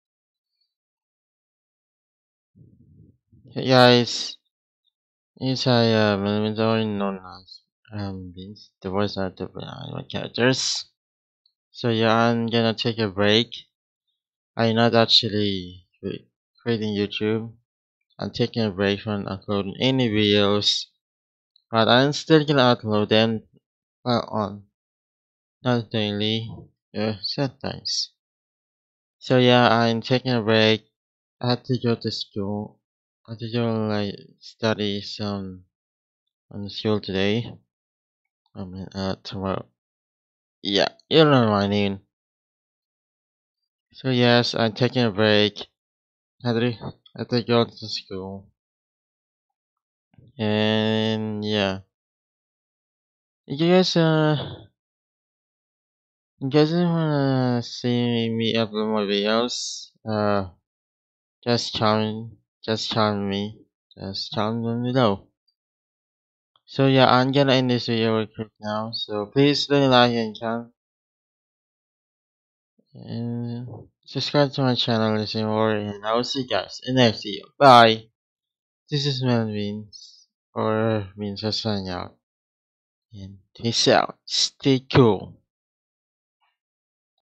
I'm taking a break.
i'm taking a break from making video and i'm be forcing on studying so yeah i still making some videos but i need to study bye